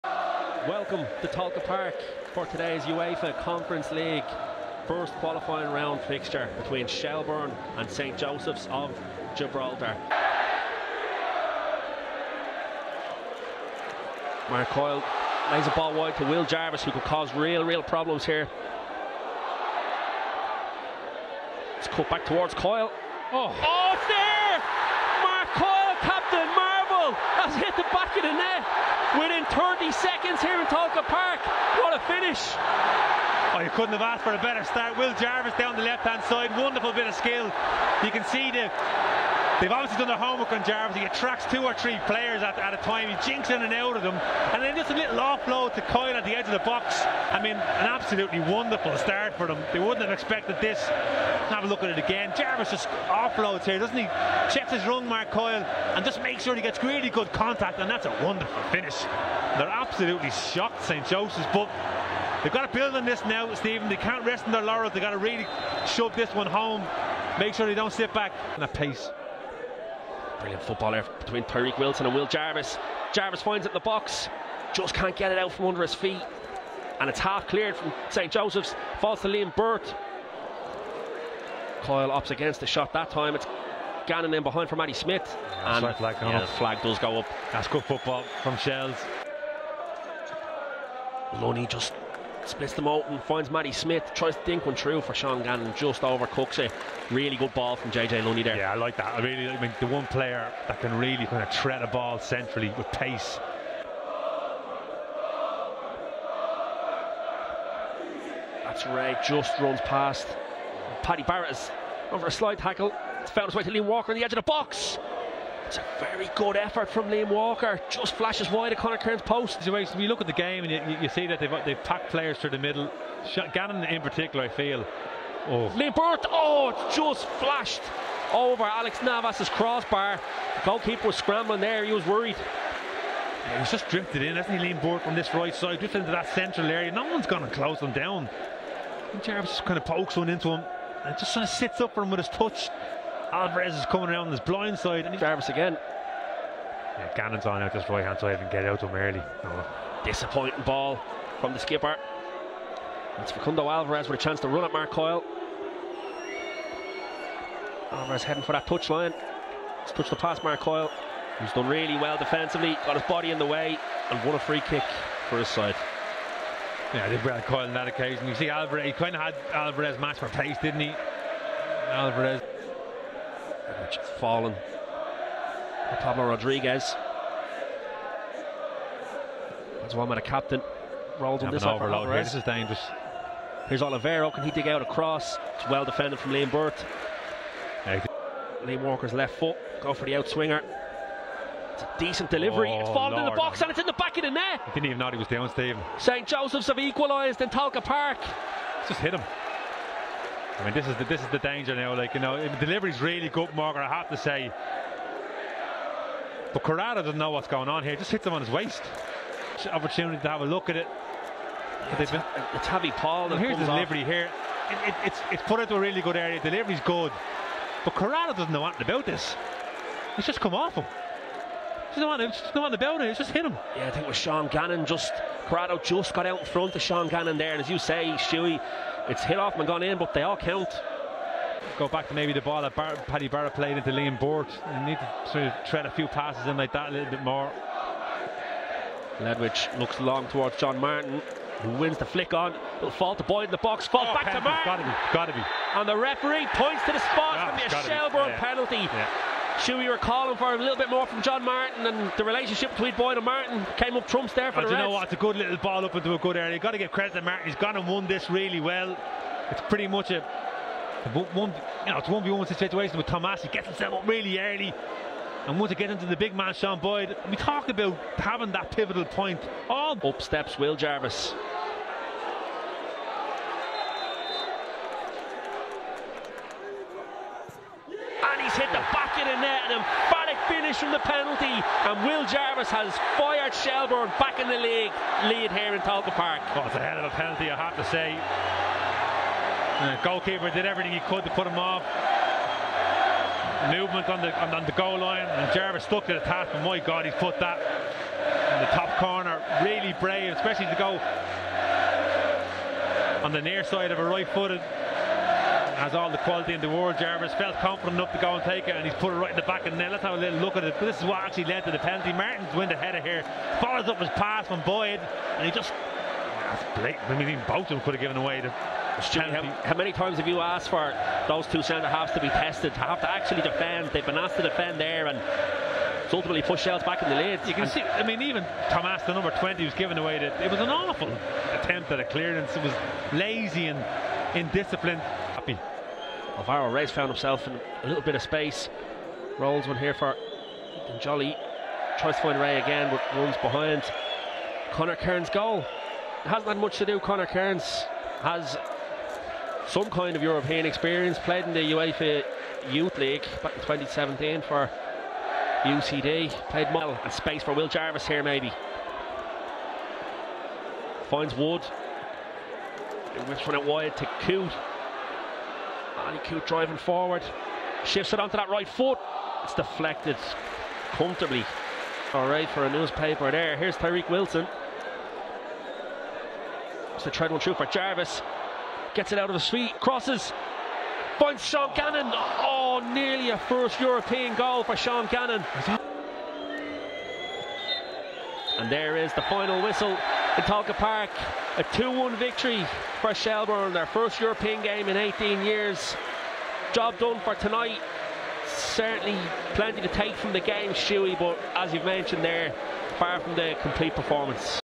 Highlights | UEFA Conference League | Shels 2-1 St Josephs
The Reds return to Europe after an 18 year exile. Tolka Park was packed to the rafters. The old lady was rocking. Commentary from Steven Doody and Stuey Byrne.